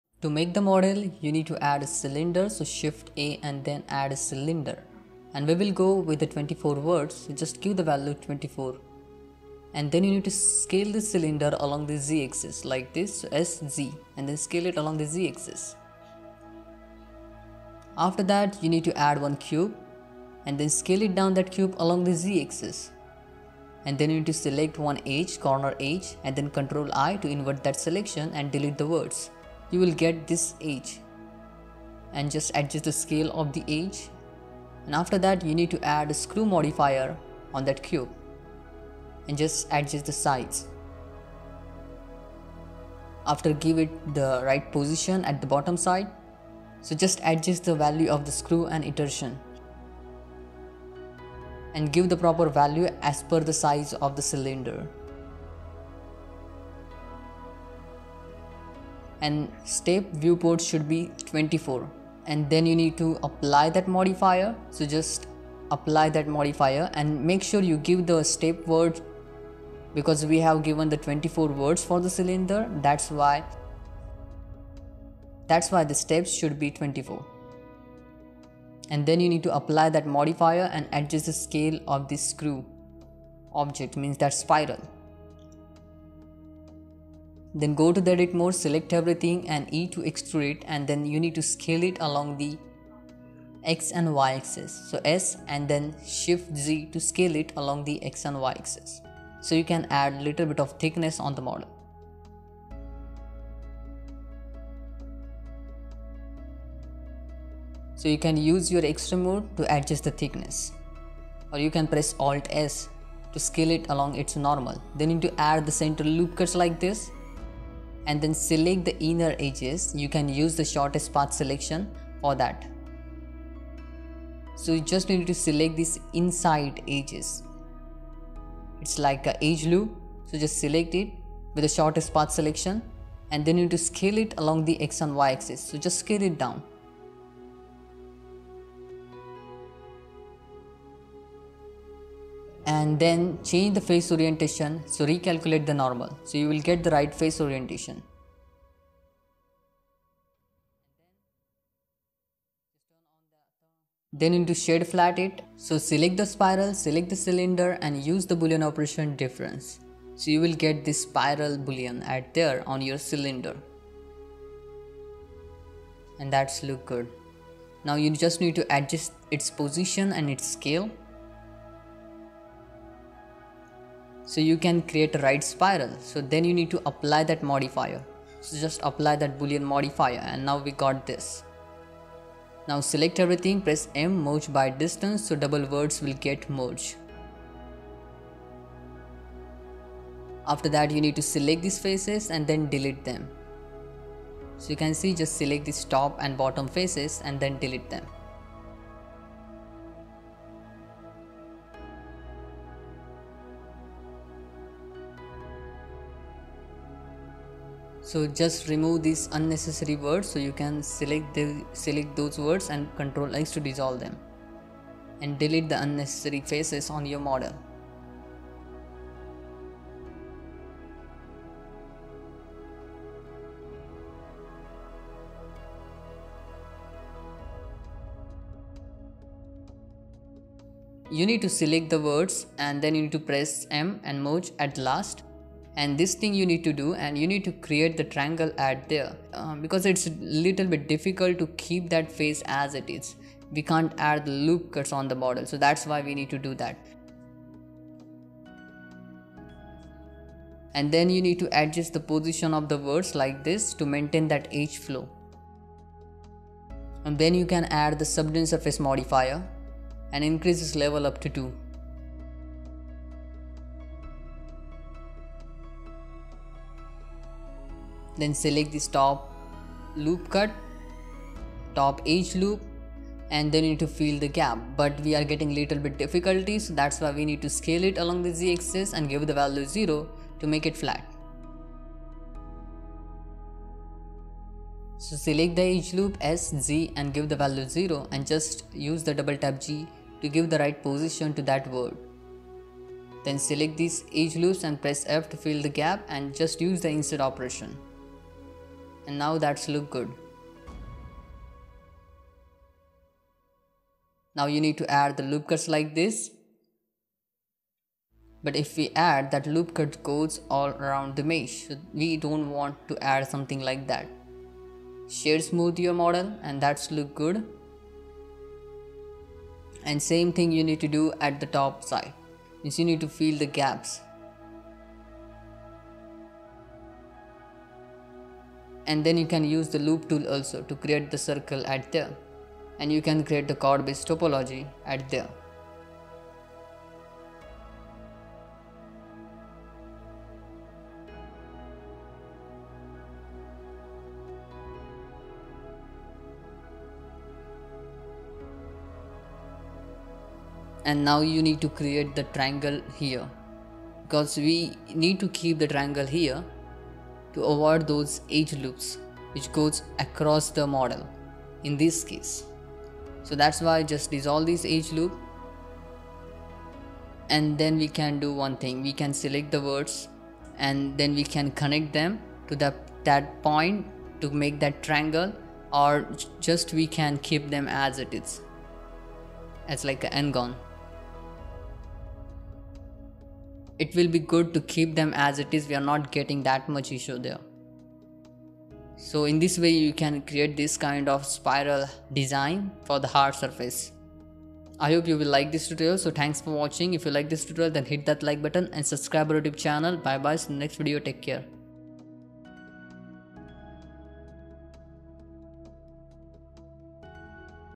Create Thread Details _ Blender 3D Modeling _ Blender 3D
In this blender tutorial I have created thread details. I have done blender hard surface modeling and I have used subdivision modeling method to make this model. I hope you will like this tutorial.

ABOUT US